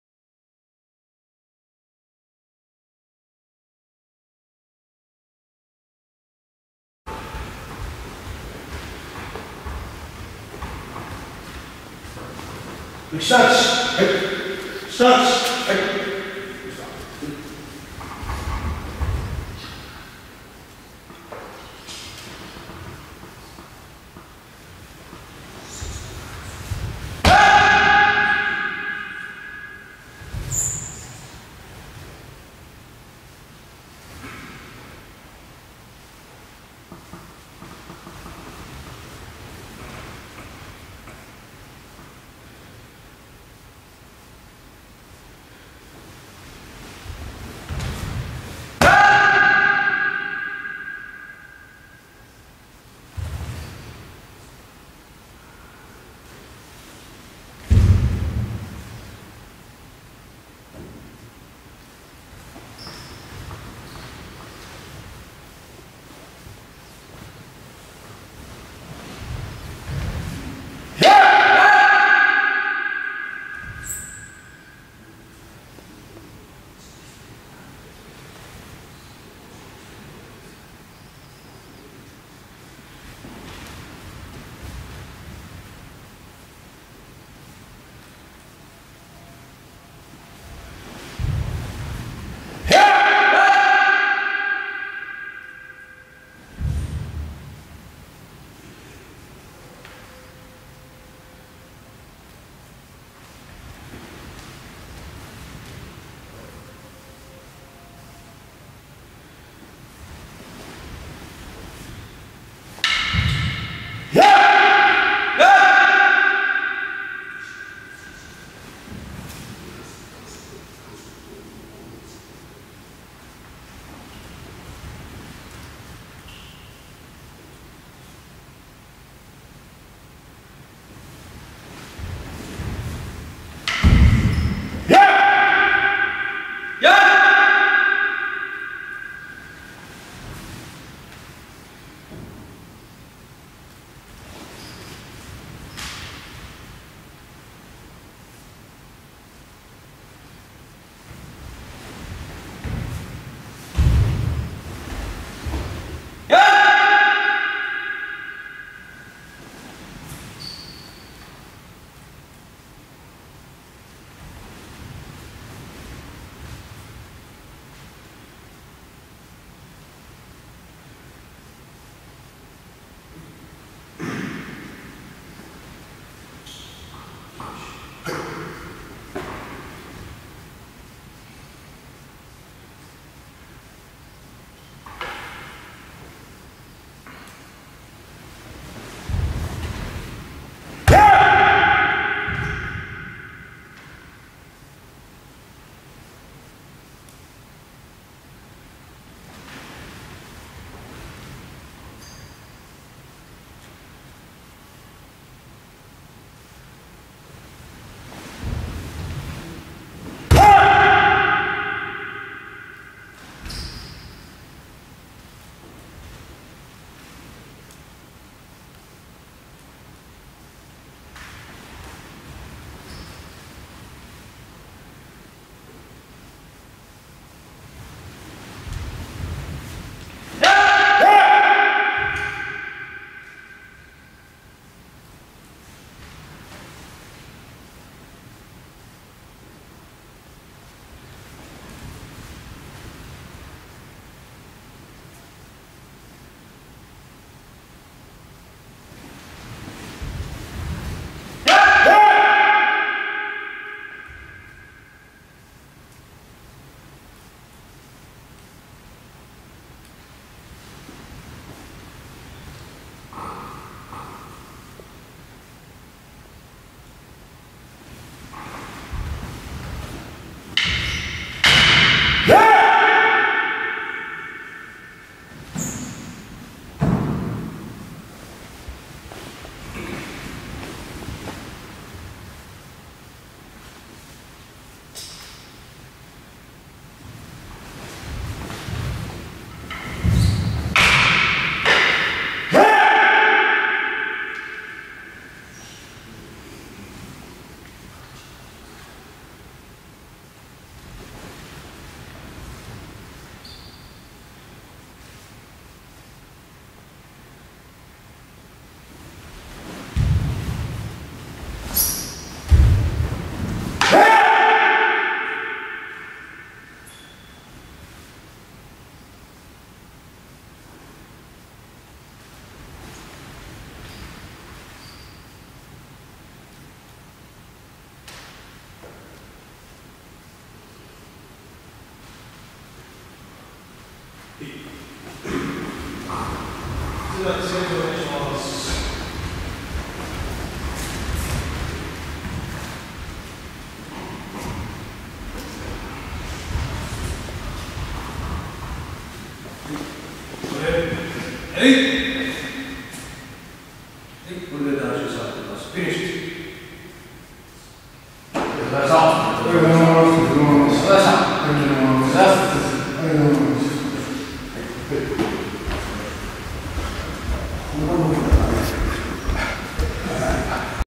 Tankendo grading (part 3)
Vladimir Kindzulis in Sendai, Japan
Tankendo grading on 3 dan
October 27, 2010
(part 3)